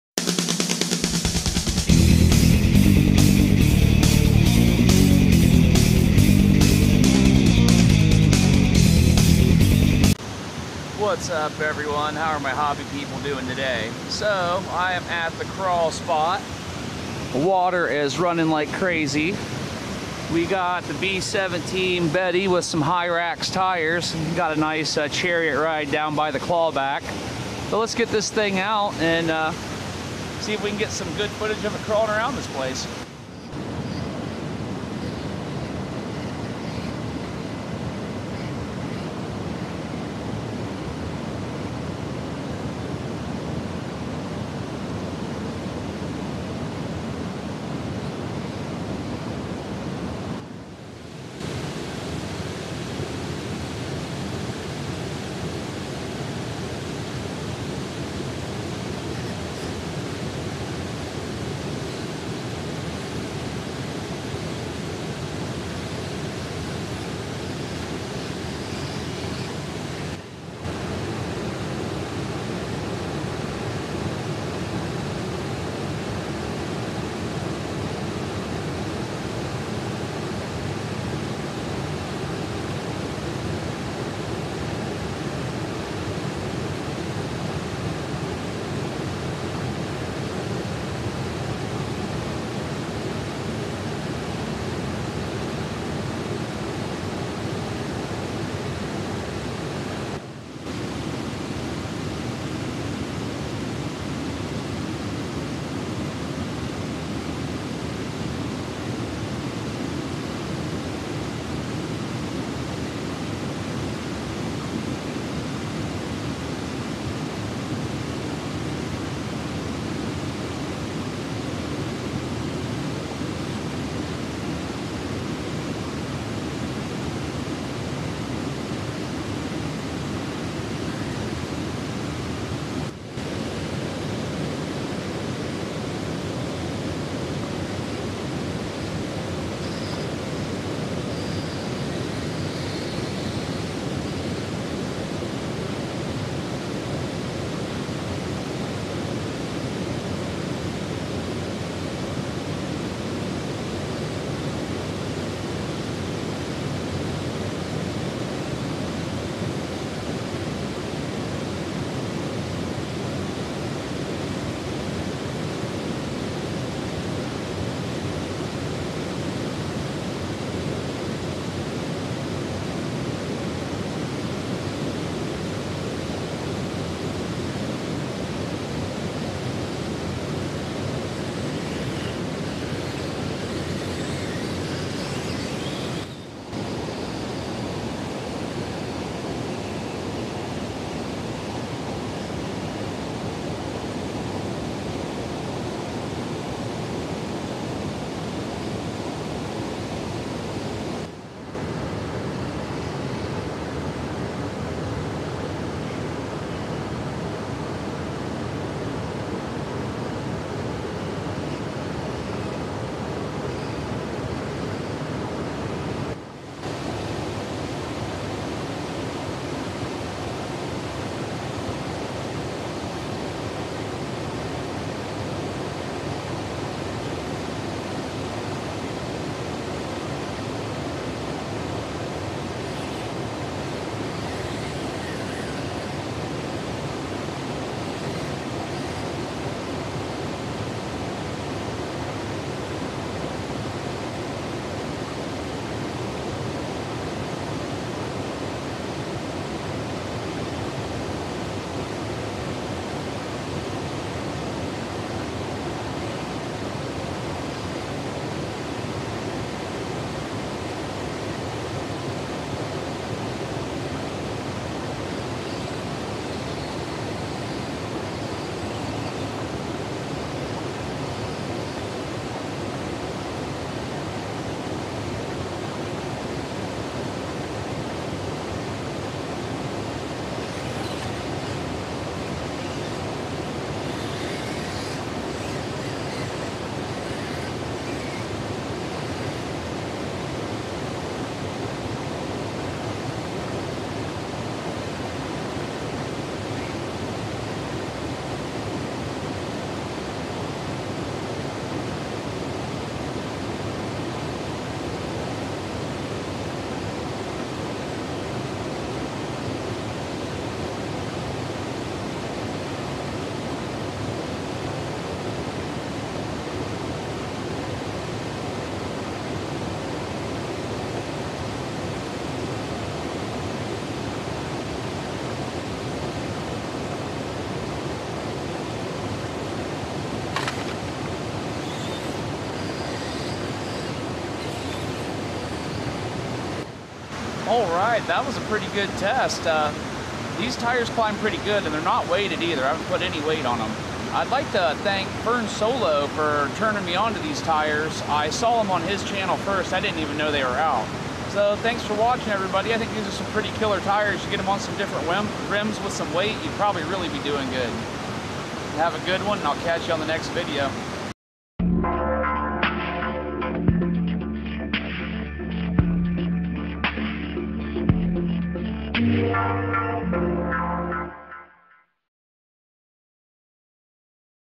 Proline hyrax 1.0 for the scx24
proline hyrax 1.0 for the scx24 proline finally got with the program and started producing 1.0 tires for the axial scx24 i put a set on the b17 Betty and headed out to try them filmed with the Insta360 Go 2 and edited with powerdirctor pro